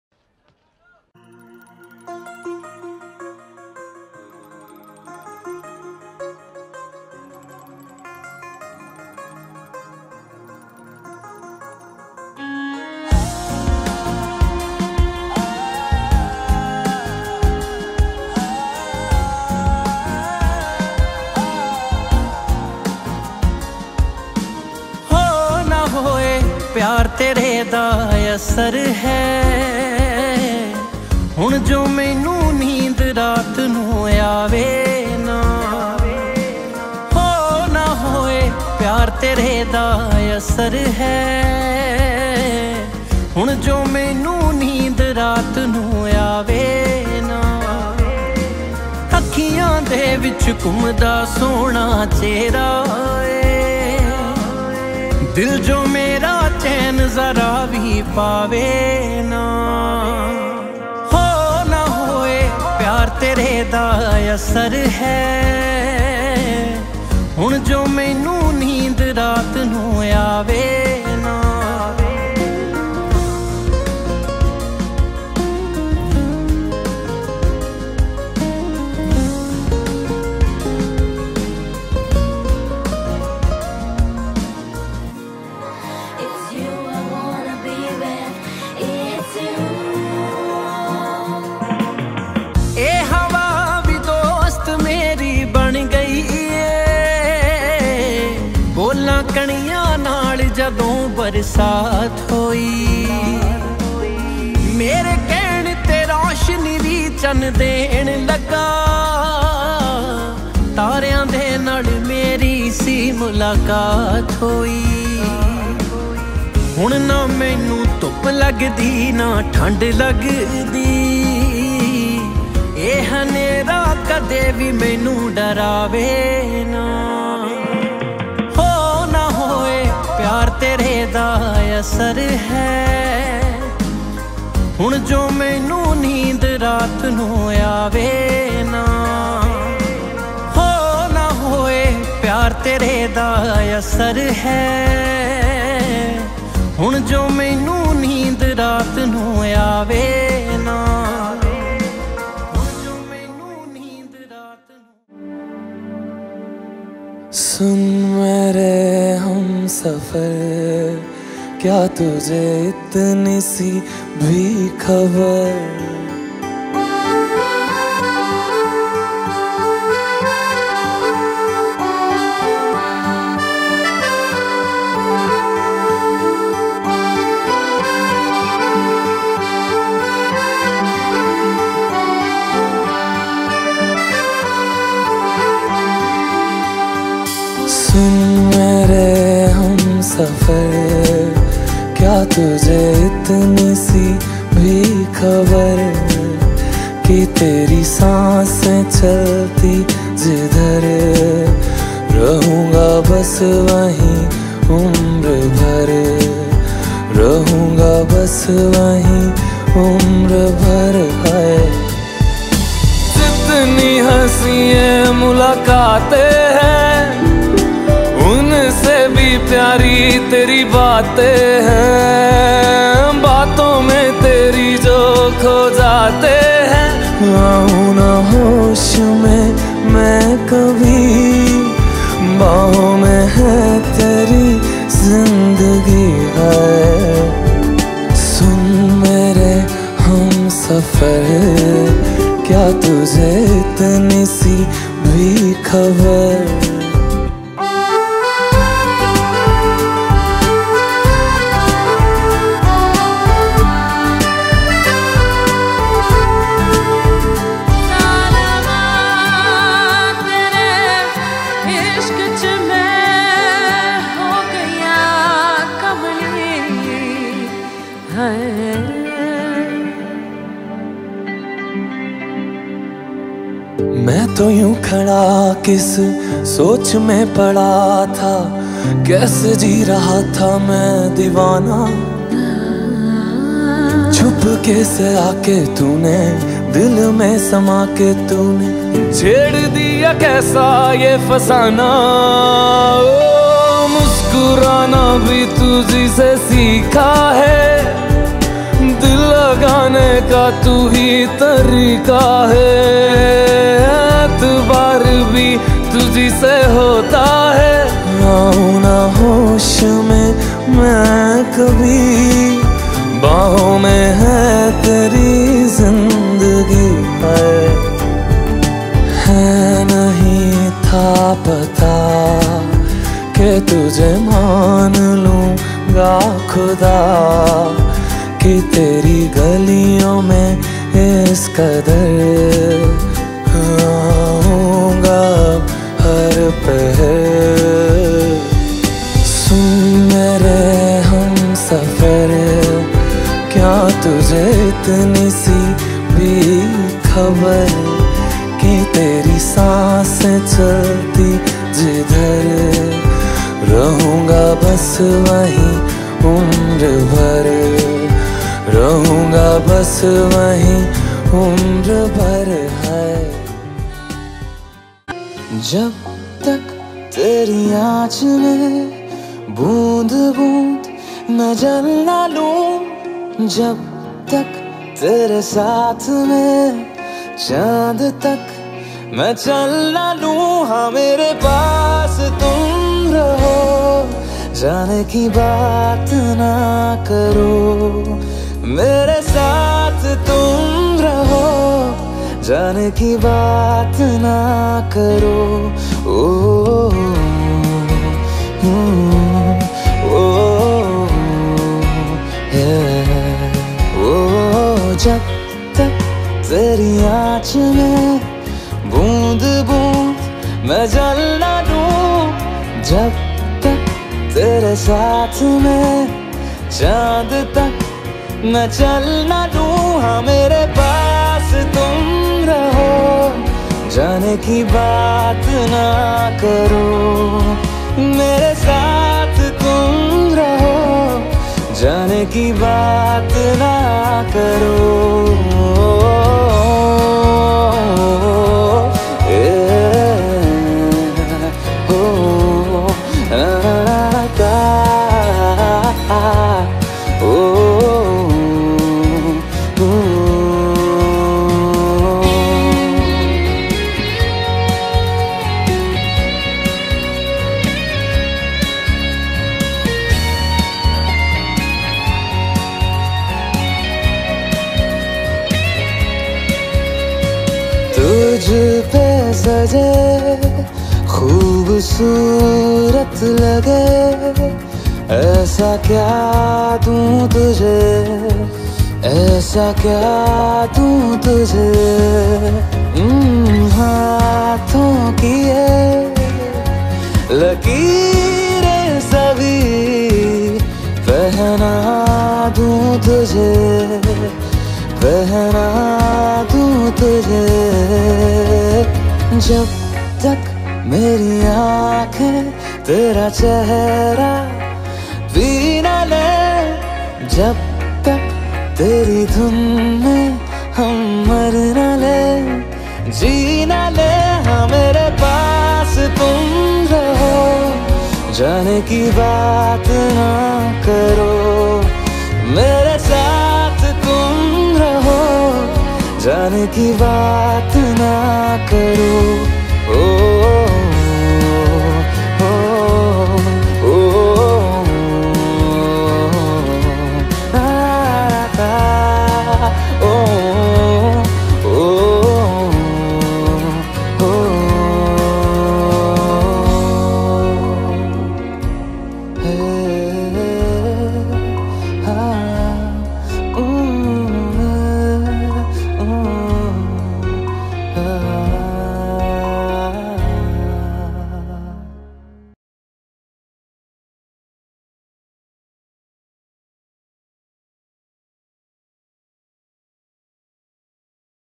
Best Indian Pre Wedding 2020 | Best DELHI Couple |Puneet & Prachi | N.K Photography
NK PHOTOGRAPHY SOLUTION

PRO- NITIN KUMAR

all types video and still wedding photography solution

EDITING BY  - BUNTY VIDEO VFX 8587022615